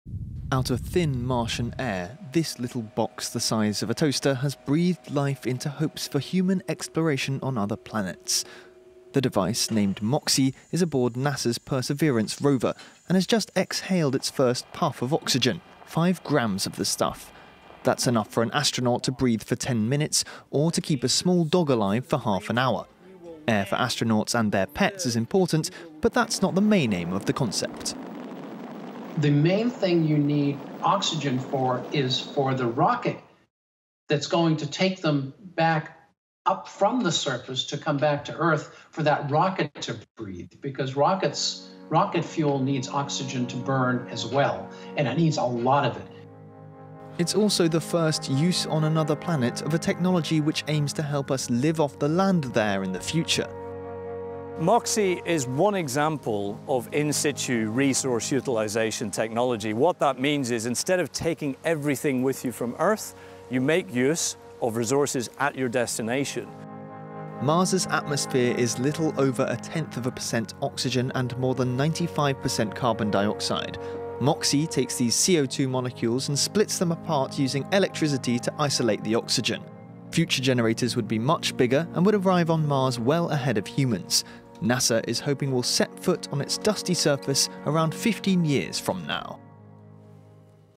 NASA extracts breathable oxygen from thin Martian air
NASA has logged another extraterrestrial first on its latest mission to Mars: converting carbon dioxide from the Martian atmosphere into pure, breathable oxygen, the US space agency said on Wednesday. The unprecedented extraction of oxygen, literally out of thin air on Mars, was achieved Tuesday by an experimental device aboard Perseverance, a six-wheeled science rover that landed on the Red Planet Feb. 18 after a seven-month journey from Earth.